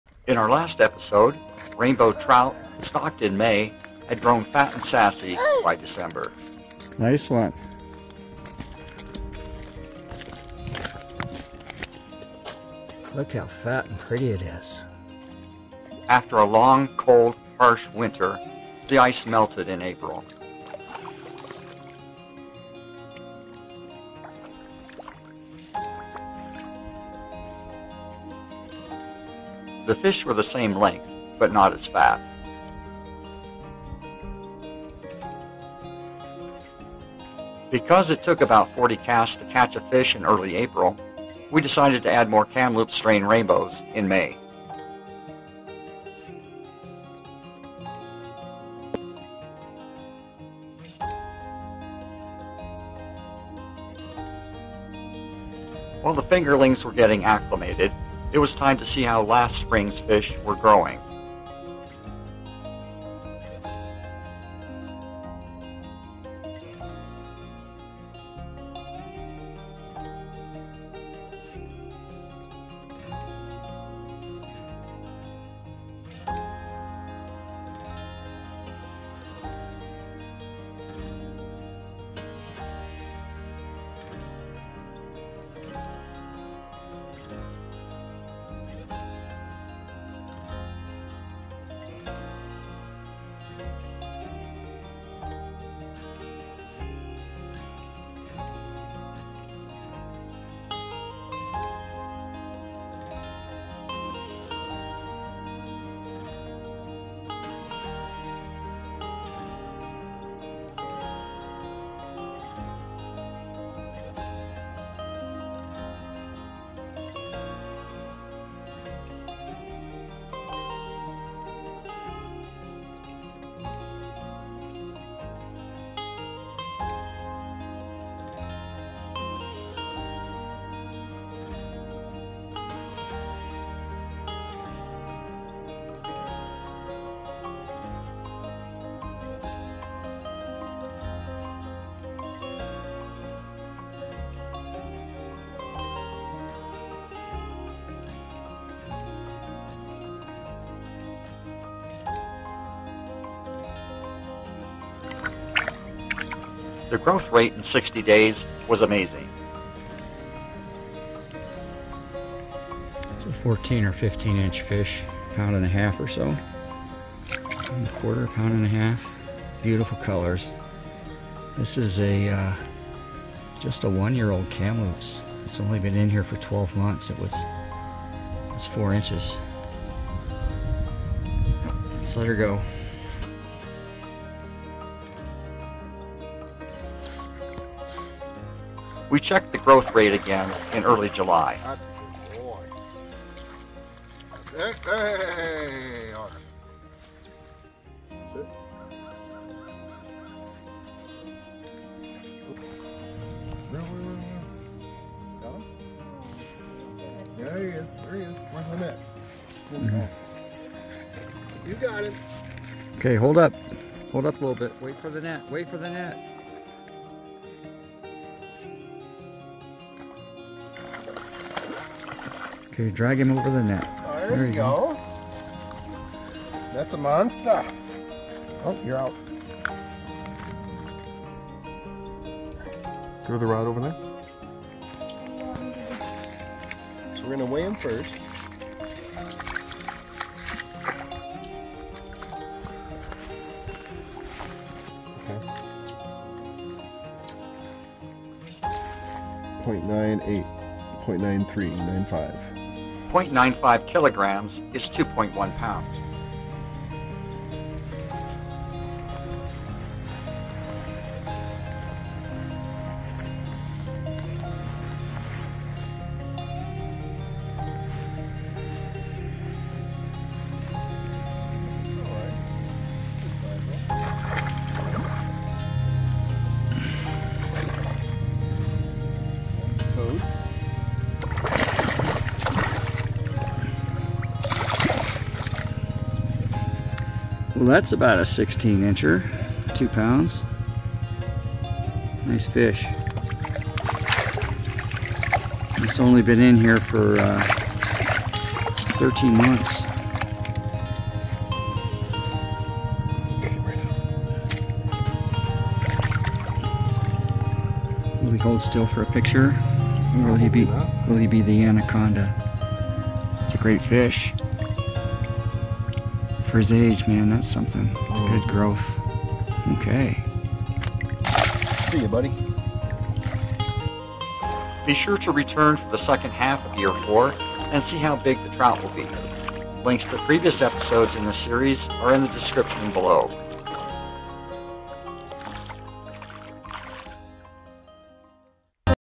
Year 4: Mid-Year Update- How Big Did Our Rainbow Trout Grow
We begin in early January and check trout growth through to July of Year 4 in our Montana pond.

The links below will show the 1st,  2nd, and 3rd year updates and videos about building and stocking our pond.